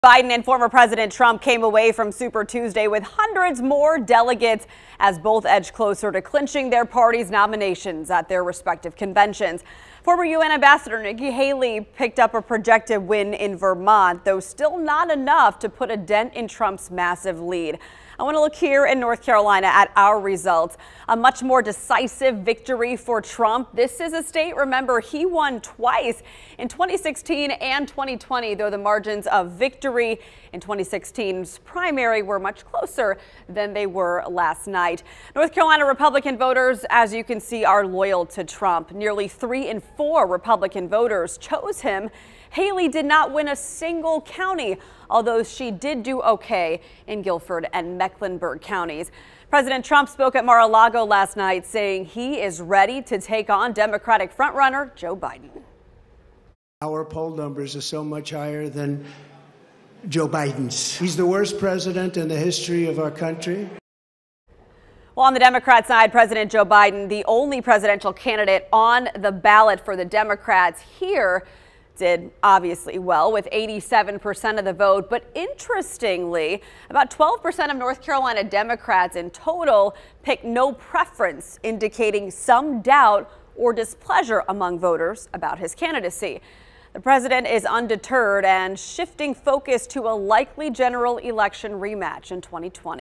Presidential Primary Breakdown
President Biden and Former President Trump came away from Super Tuesday with hundreds more delegates as both edge closer to clinching their party's nomination.